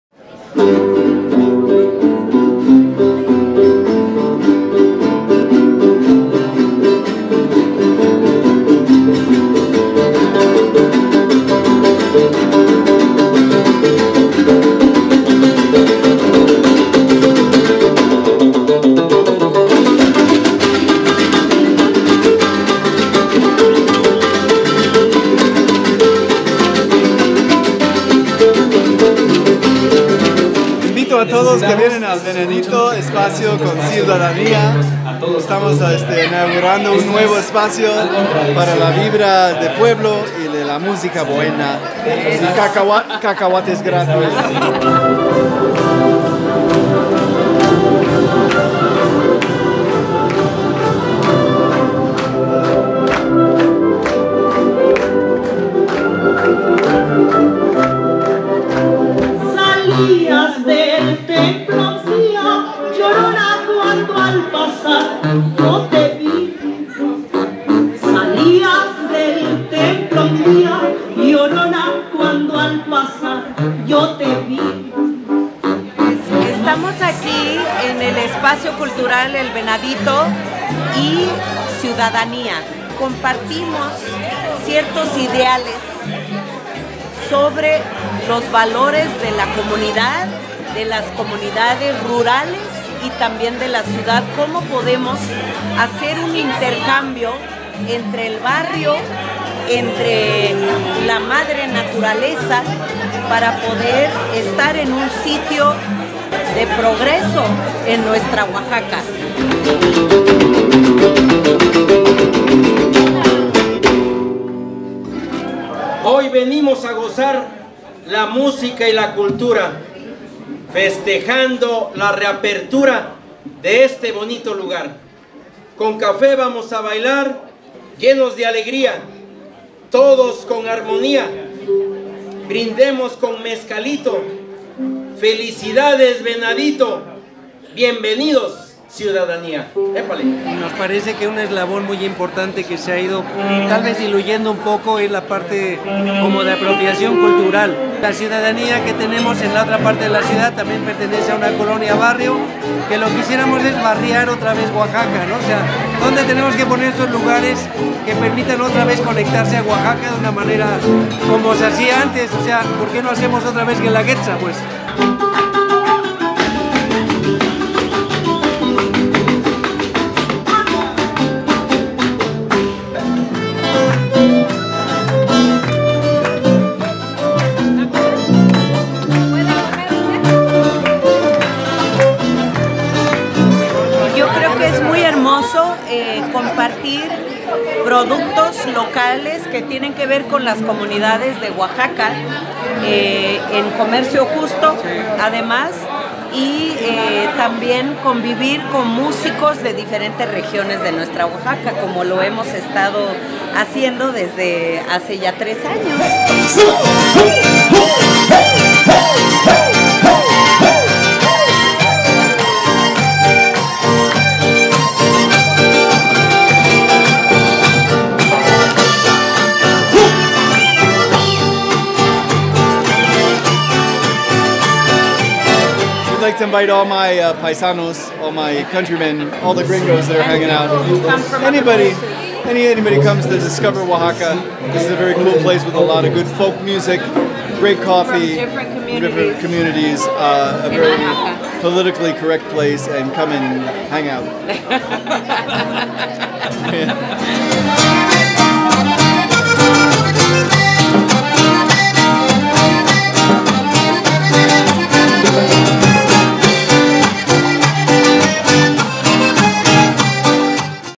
Espacio Cultural el Venadito y Café Ciudadanía
Lila Downs nos comparte la nueva propuesta de trabajo colectivo y participativo, pequeño intento para fortalecer la economía y comercio justo.
Espacio cultural el Venadito, con café vamos a bailar.